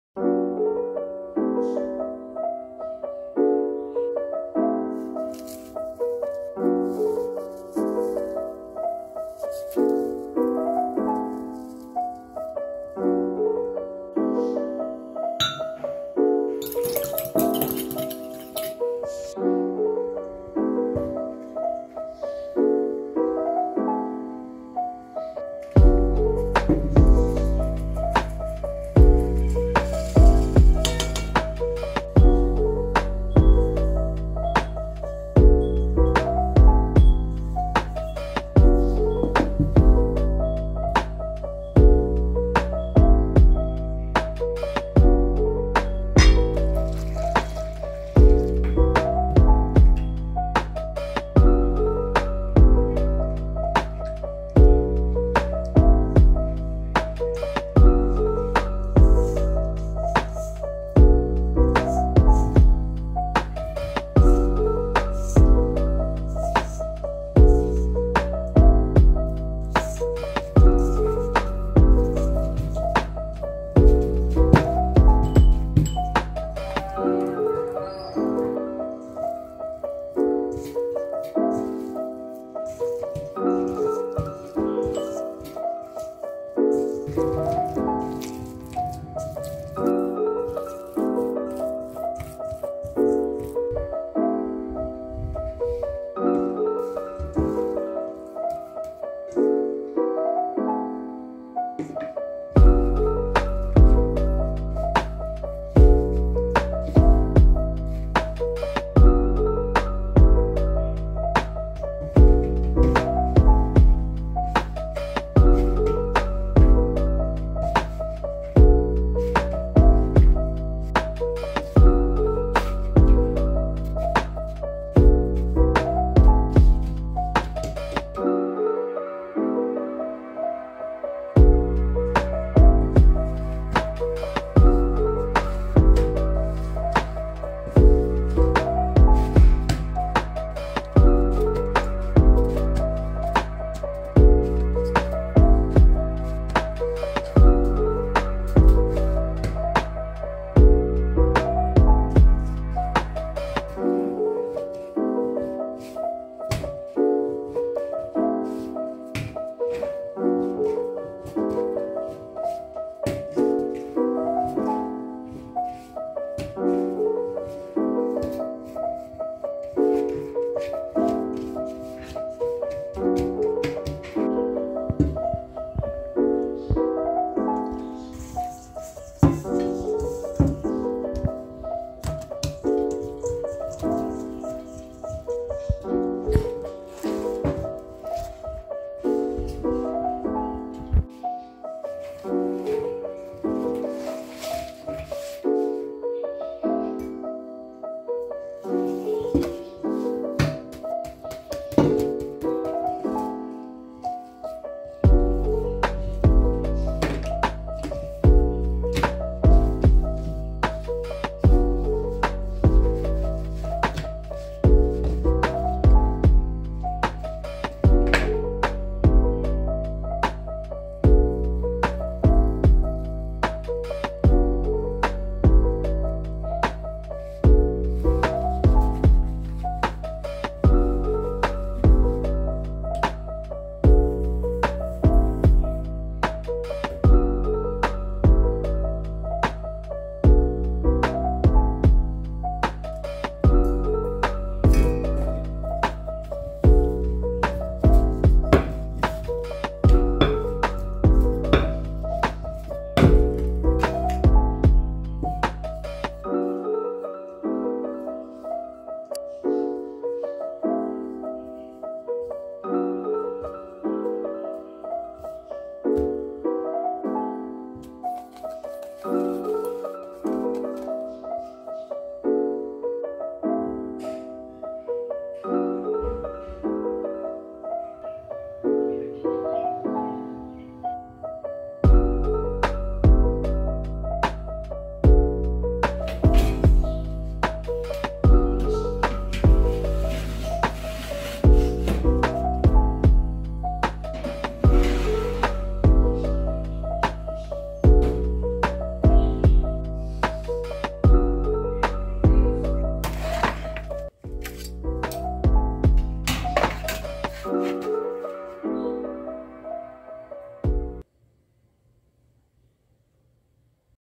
THE BEST SPANISH BREAD | MY BOYS FAVORITE!
for the filling:
1/4 cup 60g softened butter
1/2 cup brown sugar
1 tsp vanilla essence

combine all the ingridients and mix  using by
stand mixer  or whisk for 5 minutes

by: Lukrembo